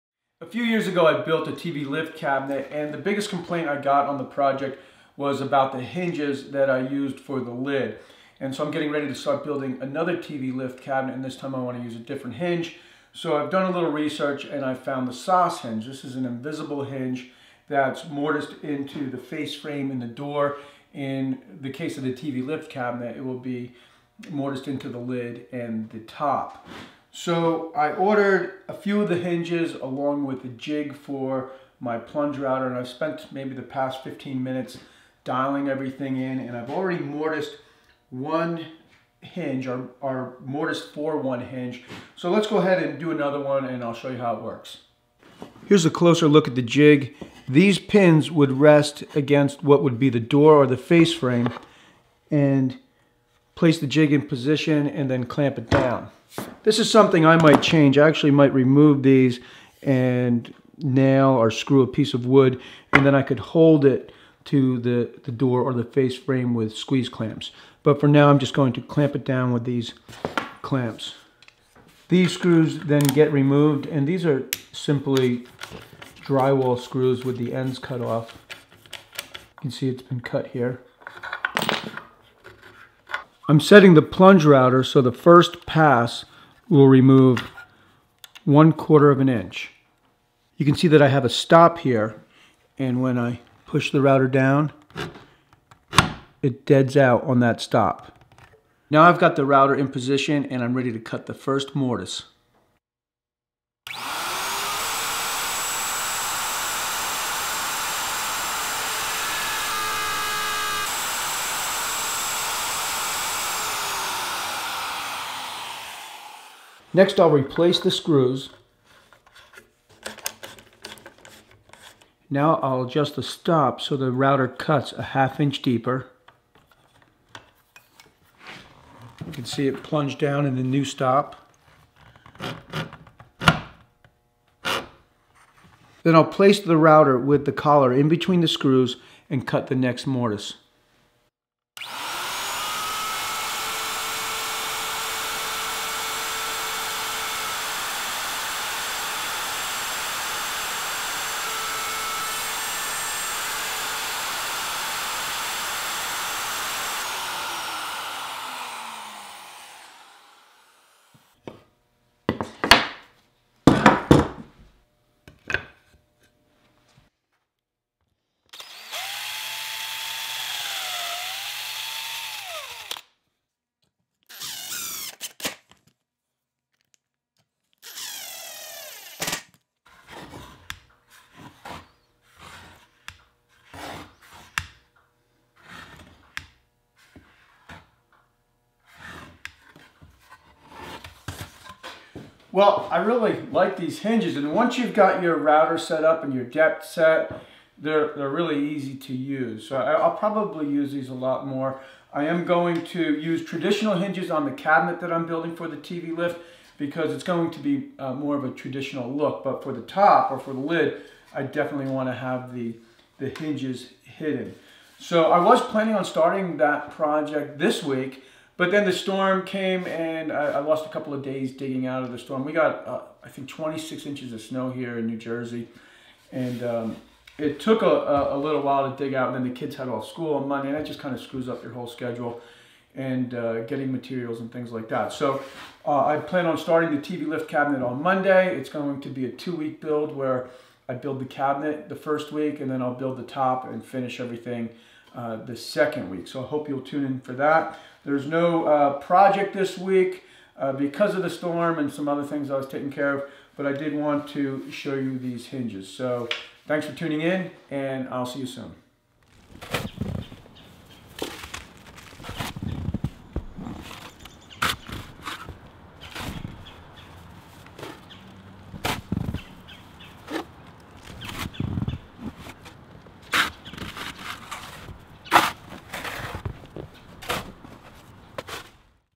EASY To Use and looks great, The Soss Invisible Hinge
Learn how to install hidden hinges for the automatic TV Lift Cabinet project I'm building. The Soss hinges are invisible and get mortised into the lid using a jig and plunge router using a 3/8" straight bit.